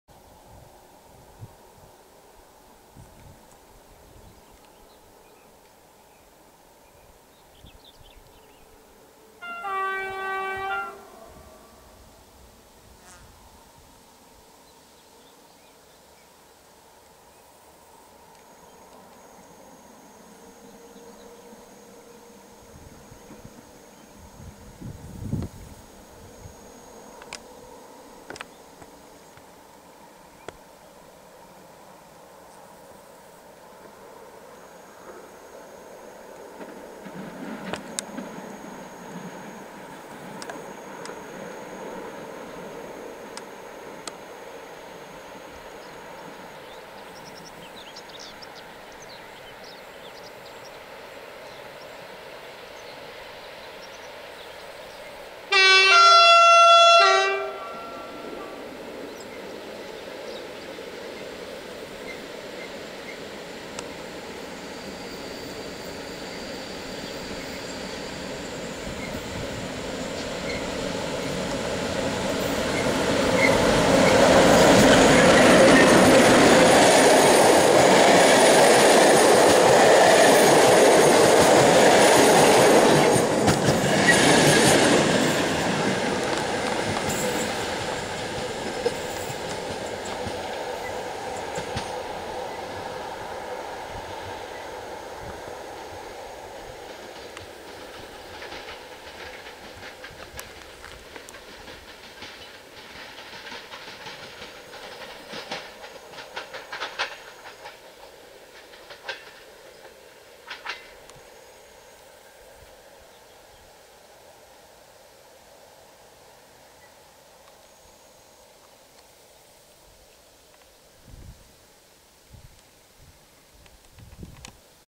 Zachodzące słońce z wracającym "Barbakanem" :)
ET22-015  z  IR 83121 "Barbakan" ze Szczecina Głównego do Krakowa Głównego przelatuje punktualnie o godz.19:48  przez p.o. Balin z ładnym Rp1 przy zachodzącym słoneczku :)
Pozdro !!!